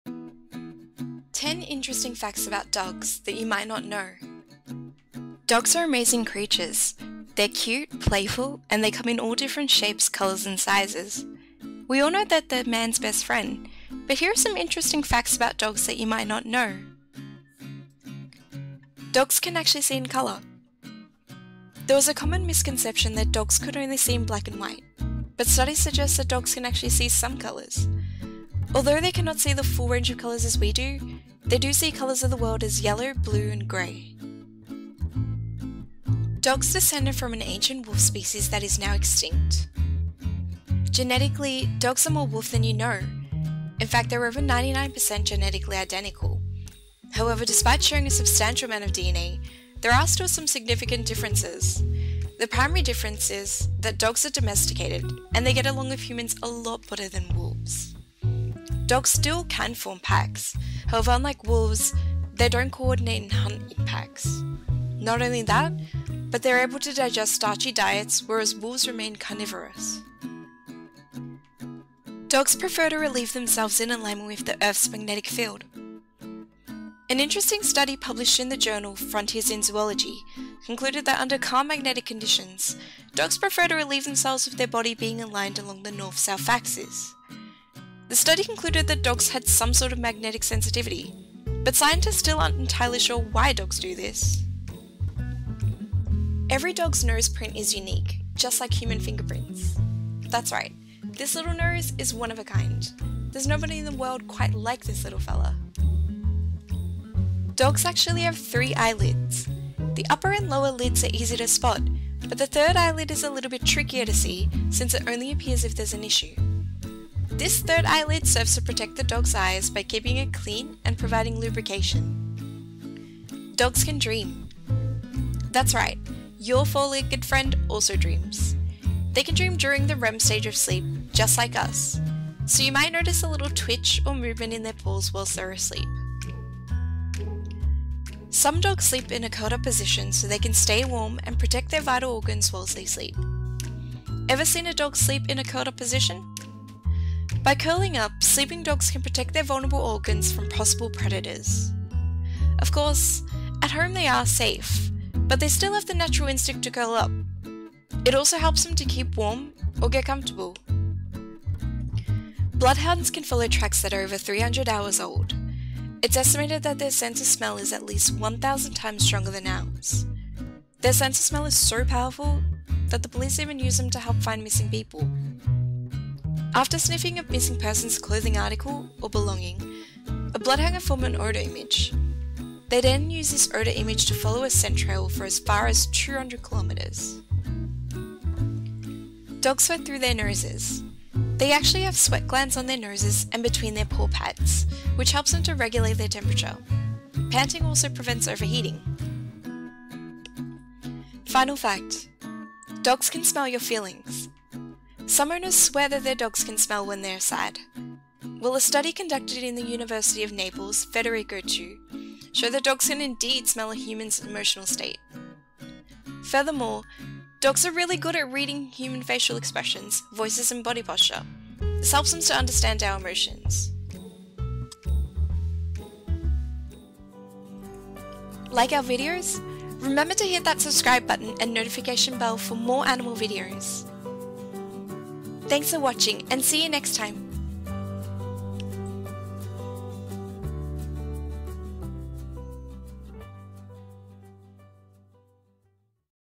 10 MOST Interesting DOG FACTS You Didn't Know
10 Interesting facts about dogs that you might not know:

1. Dogs can actually see in color.

2. Dogs descended from an ancient wolf species that is now extinct.

3. Dogs prefer to relieve themselves in alignment with the Earth’s magnetic field.

4. Every dog’s nose print is unique, just like human fingerprints.

5. Dogs have three eyelids!

6. Dogs can dream!

7. Some dogs sleep curled up so they can stay warm and protect their vital organs whilst they sleep.

8. Bloodhounds can follow tracks that are over 300 hours old!

9. Dogs sweat through their noses.

10. Dogs can smell your feelings.

Dogs being able to smell feelings (Biagio D'Aniello, University of Naples "Federico II".